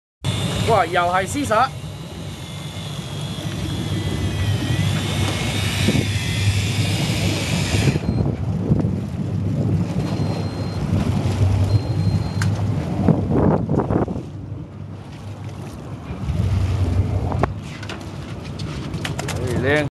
PART 3 . 09 Panama City Florida OHIOAFC Deep sea fishing DAIWA 750 MT Electric Power Assist REEL
2009 - Panama City Deep Sea Fishing Advanture , Mackerel,
Amberjack, Cobia, Dolphin, Red / Black snapper, Grouper, and many more!
DAIWA 750 MT  Electric Power Assist REEL